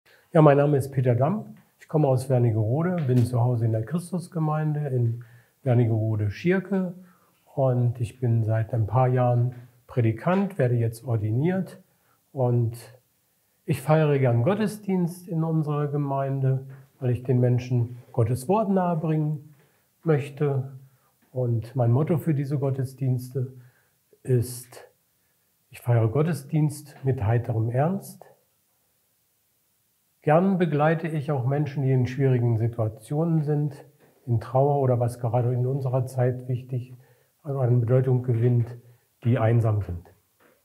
Ordination 2024: Peter Damm
Am 28. April 2024 werden die neuen Pfarrerinnen und Pfarrer sowie neue Prädikanten und Prädikantinnen der Evangelischen Kirche in Mitteldeutschland im Magdeburger Dom ordiniert. Es sind insgesamt 17 Menschen, die sich in den Dienst der EKM stellen. Einer von ihnen ist Peter Damm. Er stellt sich hier vor.